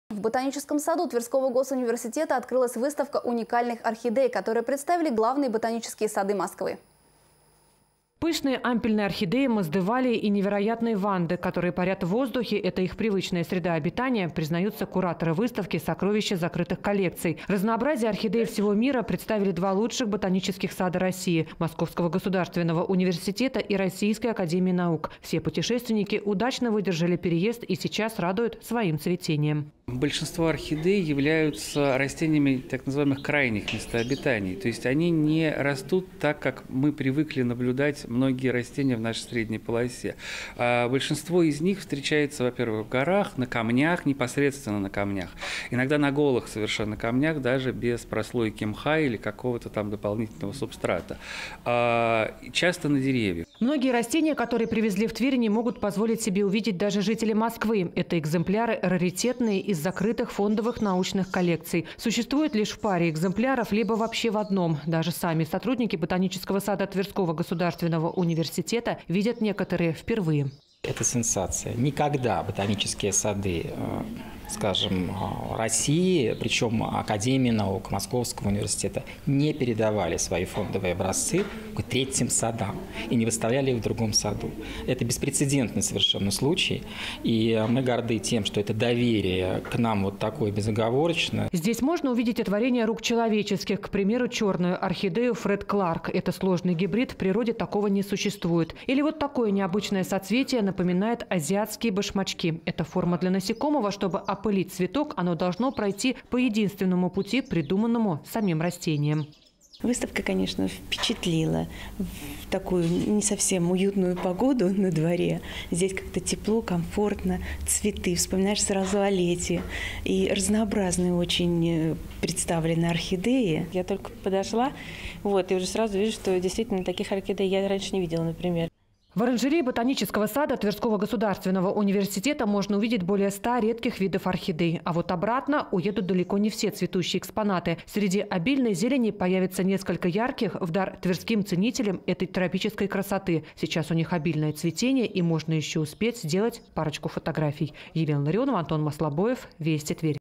В Ботаническом саду ТвГУ открылась выставка уникальных орхидей
Ботанический сад Тверского государственного университета